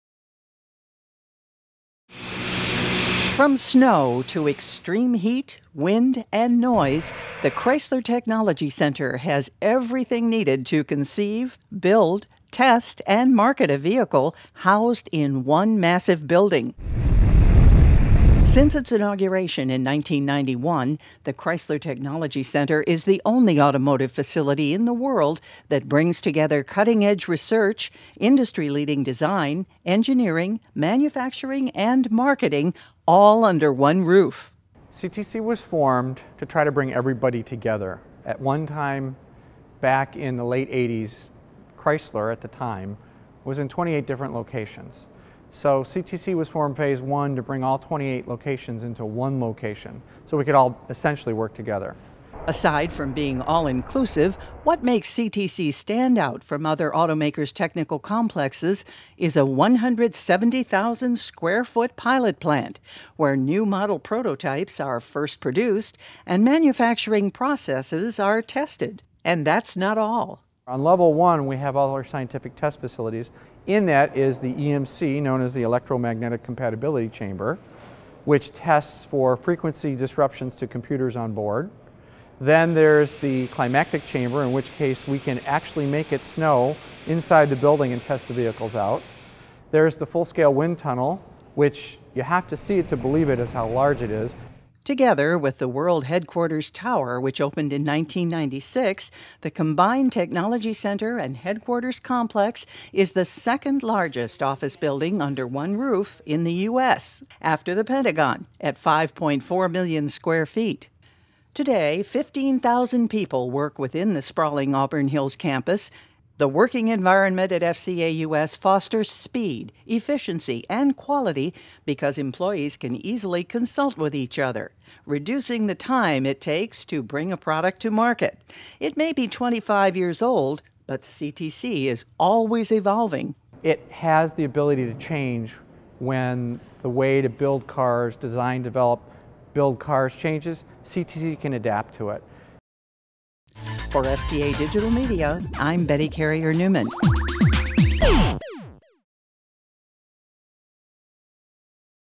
The Chrysler Technology Center: 25th Anniversary
For 25 years, the Chrysler Technology Center (CTC) has been the only automotive facility with cutting-edge research, industry-leading design and advanced manufacturing facilities, all under one roof.